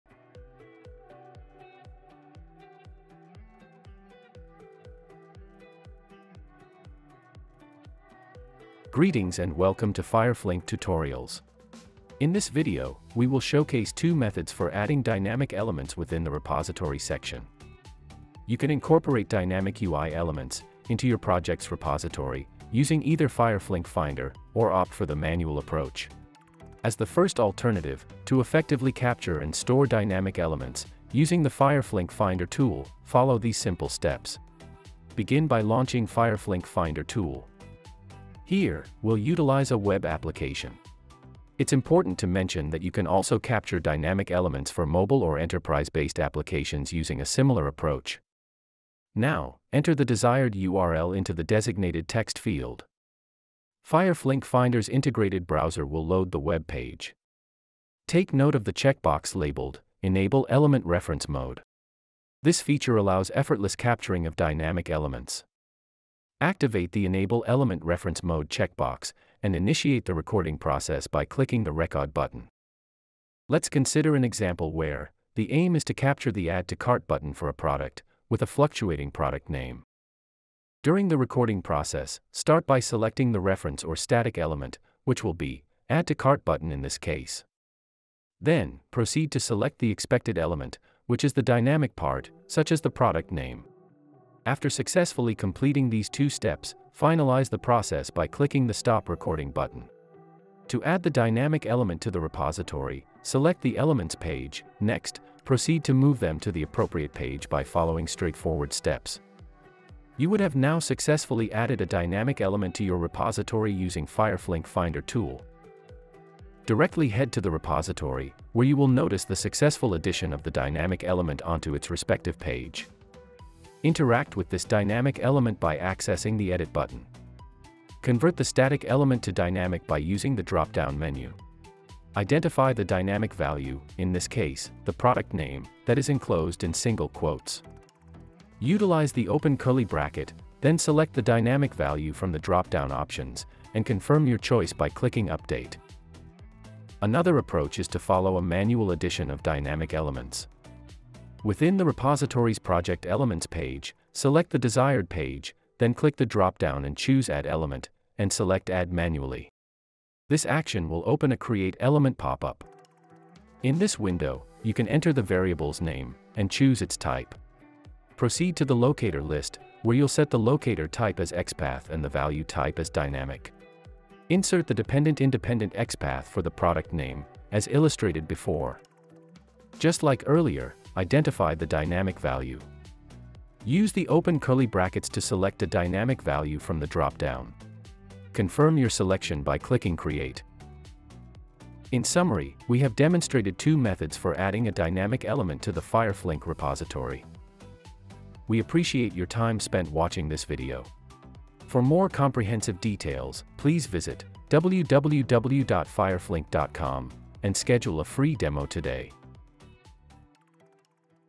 Dynamic Elements within Repository
In this video, we will showcase two methods for adding dynamic elements within the repository section. You can incorporate dynamic UI elements into your test automation project's repository, using either FireFlink Finder or opt for the manual approach.